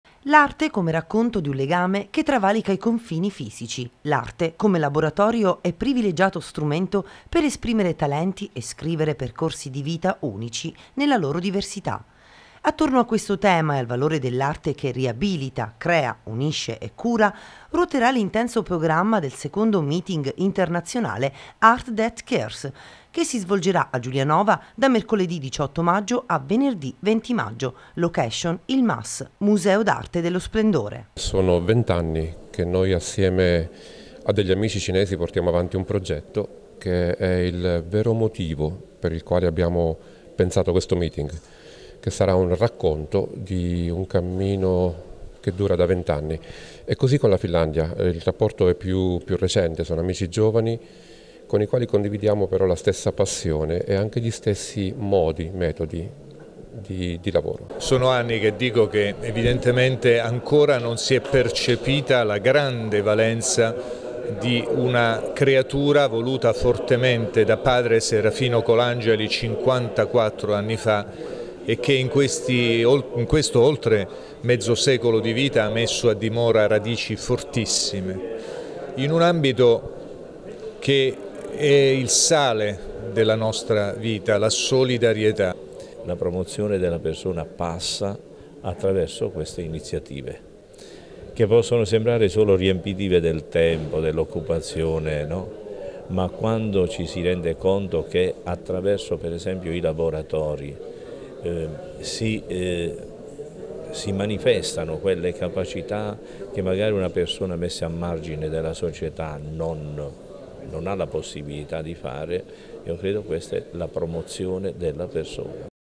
La Piccola Opera Charitas incontra Cina e Finlandia
La Piccola Opera Charitas di Giulianova incontra Cina e Finlandia con il meeting "Art that Cares" in programma da mercoledi 18 a venerdi 20 maggio